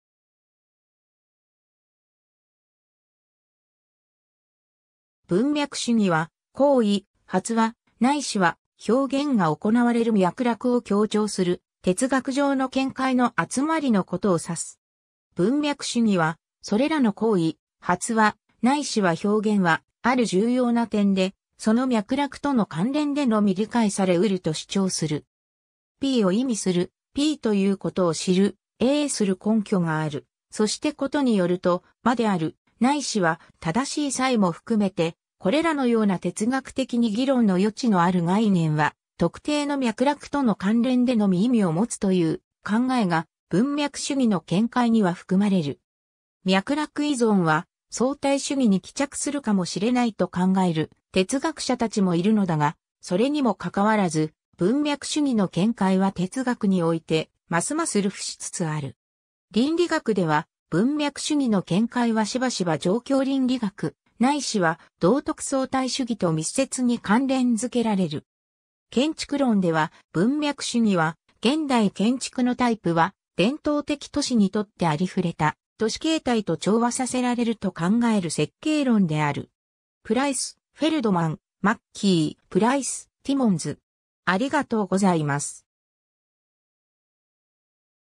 文脈主義
文脈主義（ぶんみゃくしゅぎ、英: Contextualism）は、行為、発話、ないしは表現が行われる脈絡を強調する哲学上の見解の集まりのことを指す。
文脈主義は、それらの行為、発話、ないしは表現は、ある重要な点で、その脈絡との関連でのみ理解されうると主張する。
「Pを意味する」、「Pということを知る」、「Aする根拠がある」、そしてことによると「真である」ないしは「正しい」さえも含めて、これらのような哲学的に議論の余地のある概念は特定の脈絡との関連でのみ意味を持つという考えが文脈主義の見解には含まれる。
脈絡依存は相対主義に帰着するかもしれないと考える哲学者たちもいるのだが、それにもかかわらず、文脈主義の見解は哲学においてますます流布しつつある。
 倫理学では、「文脈主義の」見解はしばしば状況倫理学、ないしは道徳相対主義と密接に関連づけられる。
 建築論では、文脈主義は、現代建築のタイプは伝統的都市にとってありふれた都市形態と調和させられると考える設計論である。
 ^ Price (2008). ^ Feldman (1999), Mackie (1977). ^ Price (2008). ^ Timmons (1998).